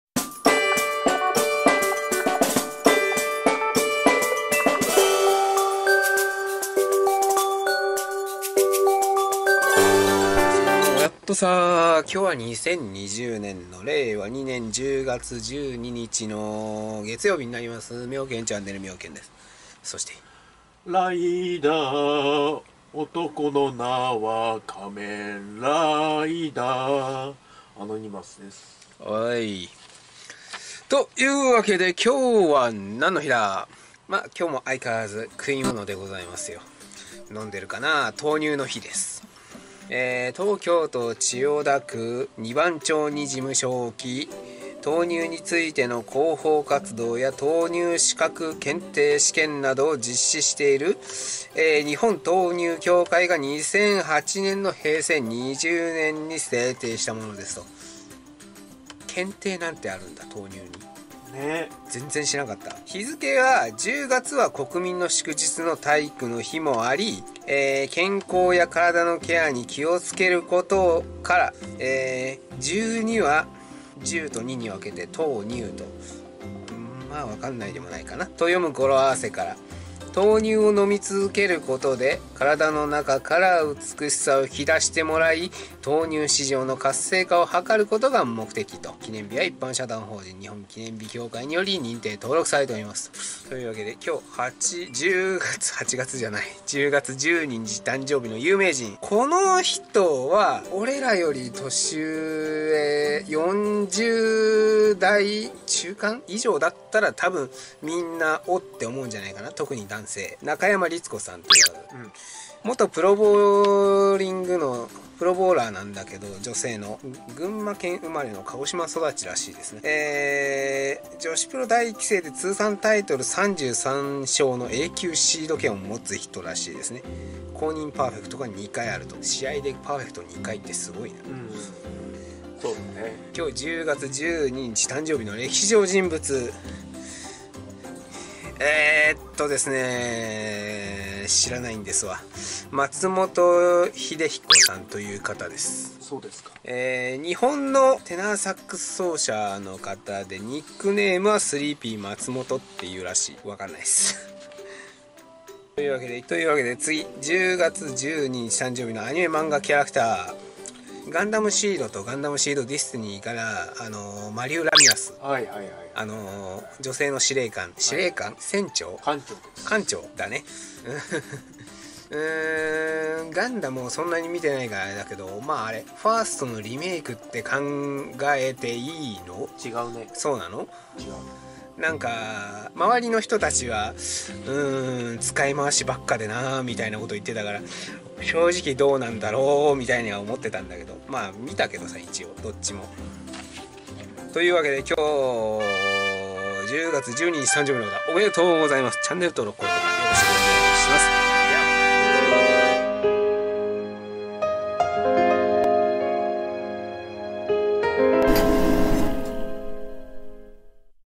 【記念日】今日は何の日？10月12日
・記念日の紹介動画です。

・今日の#誕生日
中山律子[（なかやま りつこ、1942年10月12日 - ）元プロボウリング選手。群馬県吾妻郡草津町生まれ、鹿児島県育ち。ライセンスNo.2の女子プロ1期生。通算タイトル33勝の永久シードプロで公認パーフェクト2回。社団法人日本プロボウリング協会（JPBA）会長職を4期8年務めた後、2012年3月より名誉会長。ジャパンレディースボウリングクラブ（JLBC）名誉会長。鹿児島市立鹿児島女子高等学校卒業、愛称は「和製ジャンヌ・ダルク」。弟の中山徹は、プロゴルファーでPGAシニアツアー賞金王になったこともある。]

松本英彦[（まつもと ひでひこ、1926年10月12日 - 2000年2月29日）は、日本の世界的テナーサックス奏者。ニックネームは「スリーピー松本」。享年73。甥に松本晃彦がいる。]

・使用素材
効果音ラボ

参考文献
編集#日本記念日協会、発行所#株式会社創元社
・今日は何の日～毎日が記念日～